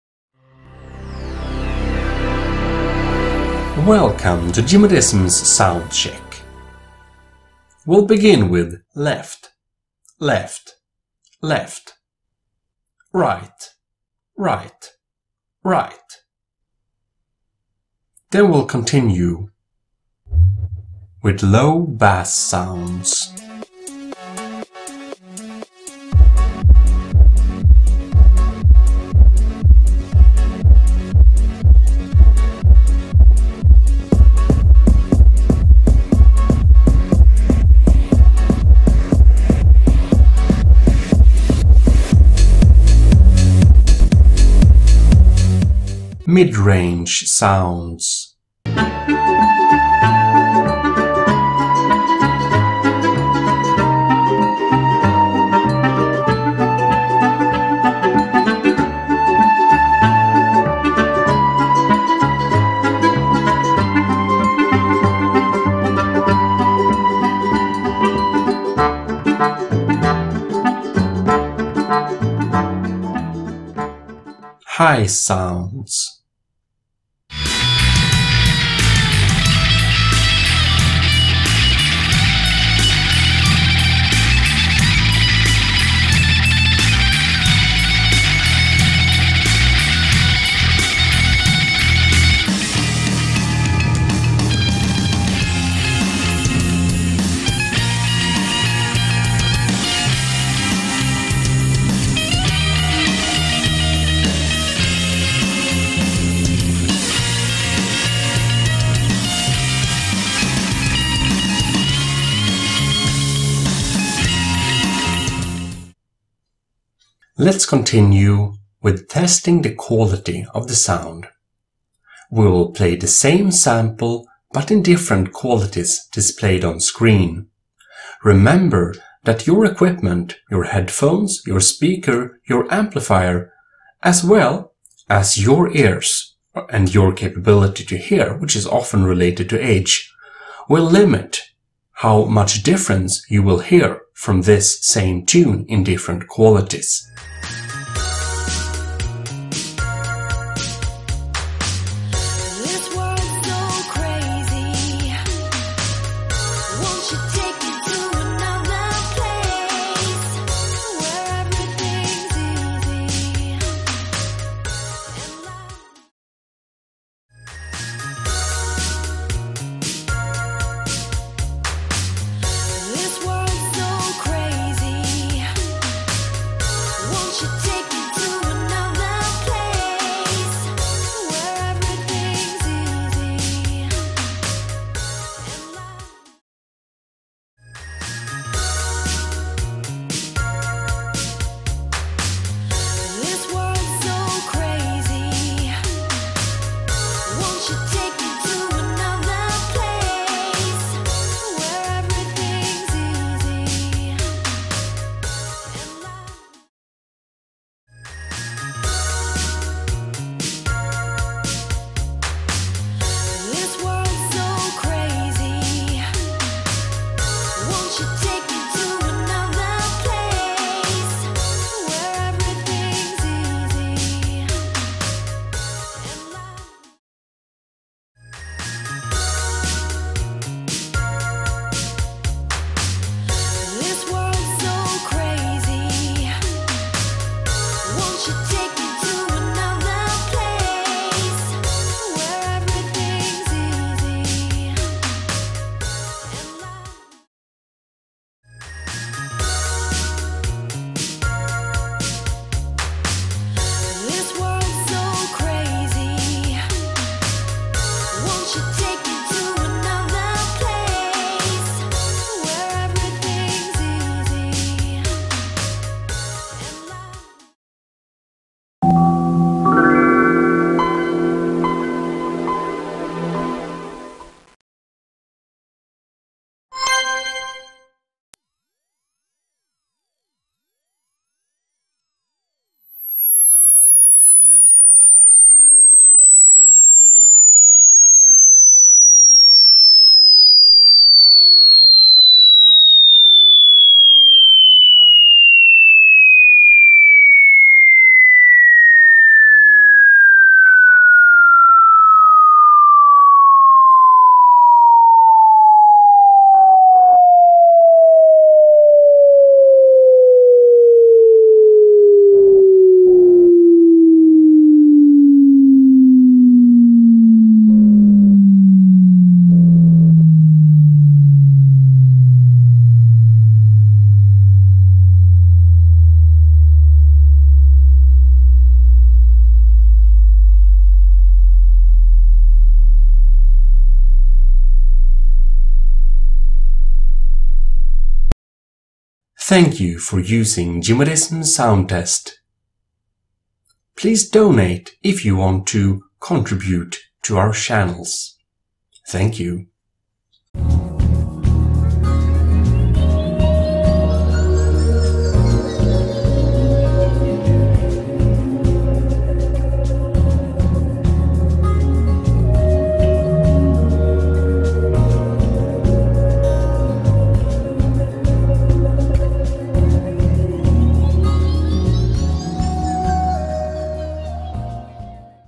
Test Your Speakers/Headphone Sound Test: Low/Mid/High, L/R Test, Bass Test, Quality, Frequency Range
This compact test let's you test your speakers quality and sound capability. Tests headphones audio quality as well as speakers and tests your amplifier. This sound test checks sound reproduction in low and bass, mid and high sounds, and that you have speakers left right test properly set up. It also tests your sound for sound reproduction by testing the entire audible range for humans.

00:00 Intro (HD version in description)
00:08 Left / Right Sound Test (Yes I am very sure this is correct L/R orientation)
00:20 Bass Boosted Test (should be no distortion) Watch HD version instead description!
00:46 Mid Range Test (Should sound clear and nice) WATCH HD VERSION
01:14 High Range Test (You should hear all drum details articulated and clear)
01:56 Sound Quality Test (Does your amp, sound card and headphone deliver all the way?)
04:28 Frequency range test (should be no distortion in the sweep) Also, don't blast your pets!
5:50 WATCH HD VERSION INSTEAD! ;) Outro (watch how to improve Windows 10 sound quality in description)

★The bass test/ low band test should sound deep and not distorted
★The Mid band test should sound clear and nice
★The High band test/ treble sounds should sound very clear, articulated and light, not distorted or blurry or messy, you should hear high drum sounds clearly.
★The Frequency band test should sound like a clear and nice sine wave curve all of the sounds you can hear.
★For the quality test, it mostly checks that your amplifier does not destroy your sound, if you have a cheap Class D anmplifier it might mess with your sound, to do this full test download this test here (this exact video but with better audio than YouTube can have) (Fixed encoding issue, this downloadable is high definition)

Hope this sound / audio test  video will help you.
Top donations are displayed in the hall of fame on my twitch page.

Licensed to me according to our agreements.